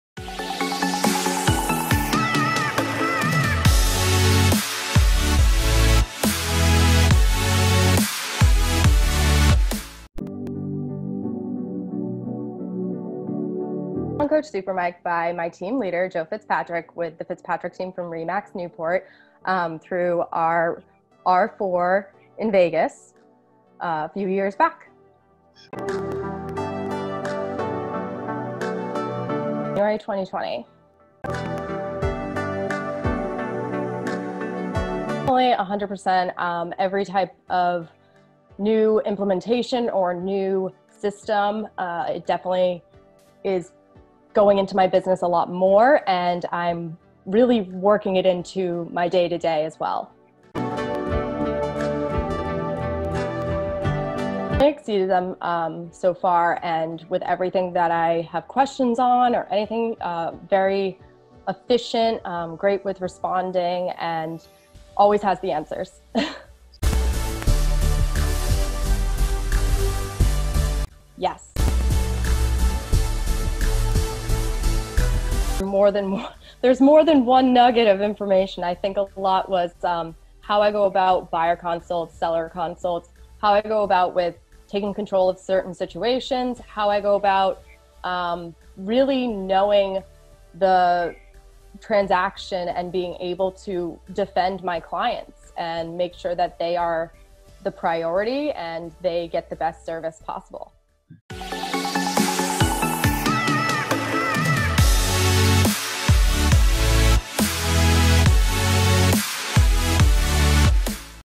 Hilary Marshall reviews "Coach Super Mike"
Hilary Marshall of The Fitzpatrick Team at RE/MAX Professionals of Newport reviews the services of "Coach Super Mike"!